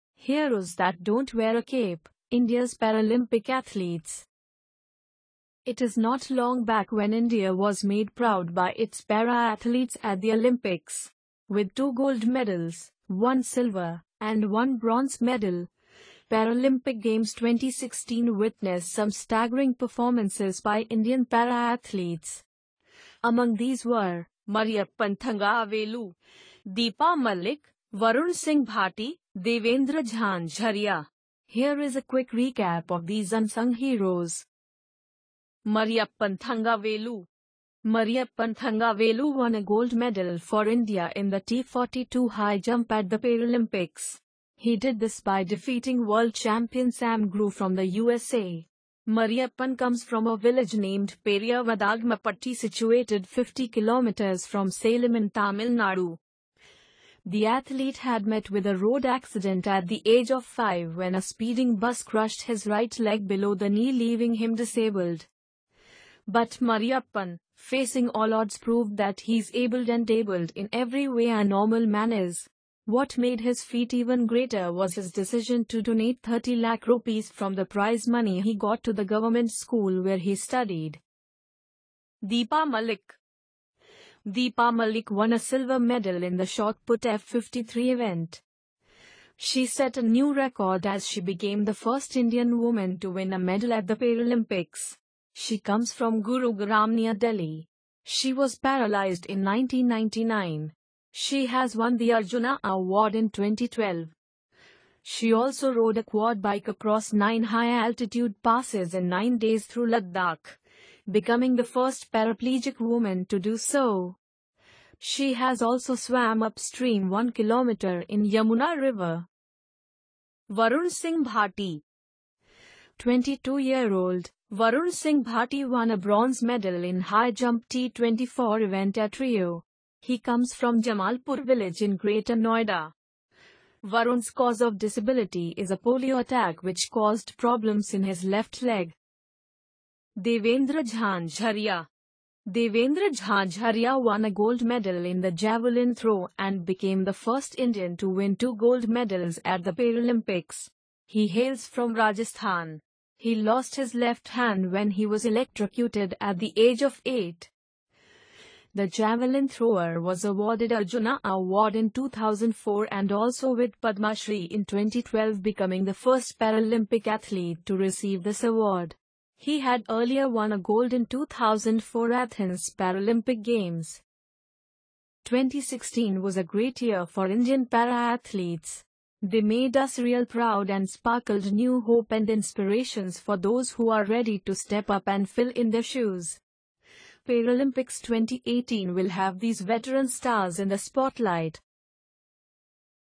Heroes that don’t wear a cape: India’s Paralympic Athletes
It is not long back when India was made proud by its Para Athletes at the Olympics. With two gold medals, one silver, and one bronze medal, Paralympic Games 2016 witnessed some staggering performances by Indian Para-athletes. Among these were, Mariyappan Thangavelu, Deepa Malik, Varun Singh Bhati, Devendra Jhajharia. Here is a quick recap of these Unsung Heroes: